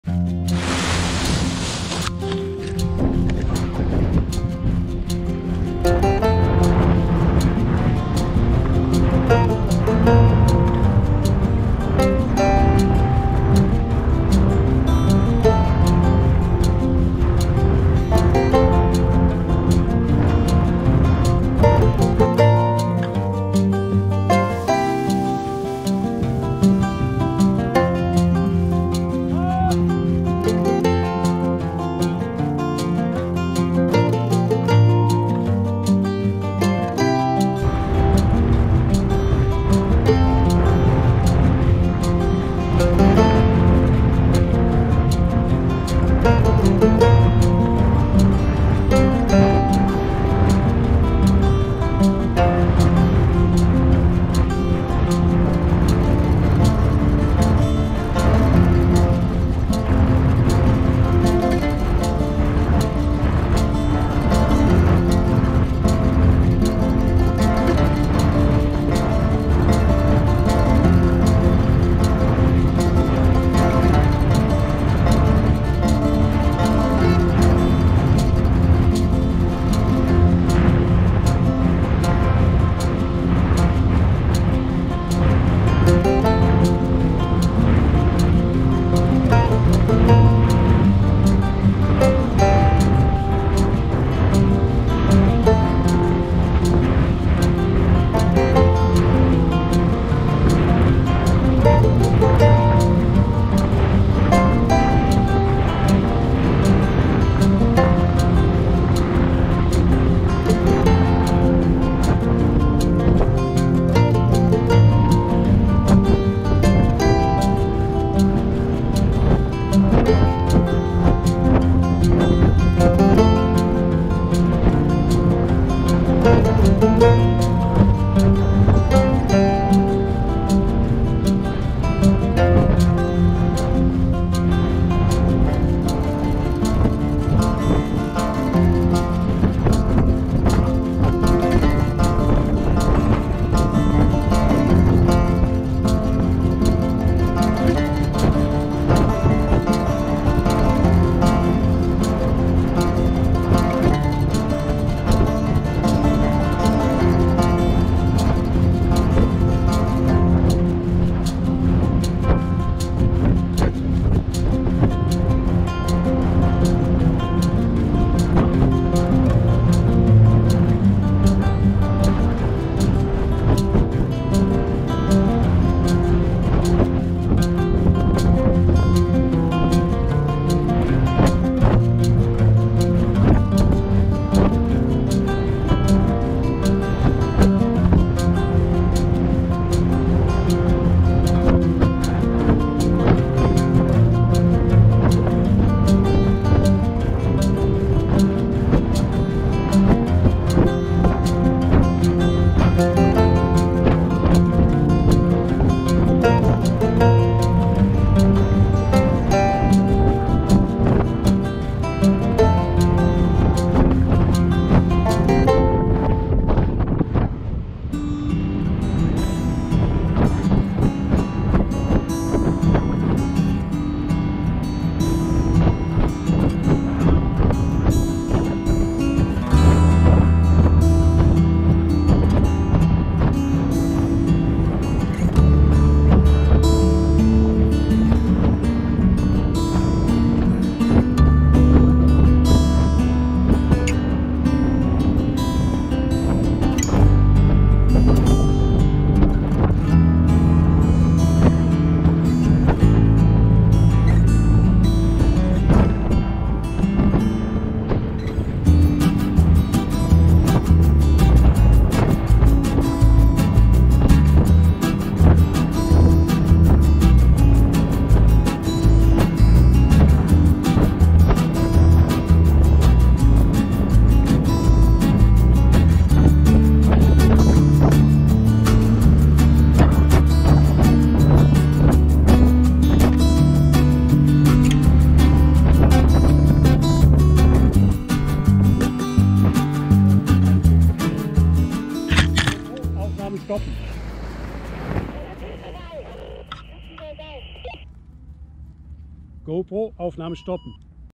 Gleitschirm Windenstart mit ChaseCam in Montabaur
Flug im Spätsommer mit ChaseCam. Den Start von unten gefilmt hat Anika. Danke dafür :)

Gleitschirm: NOVA Prion 4
Chase Cam: PPG Smoke
CAM: GoPro7black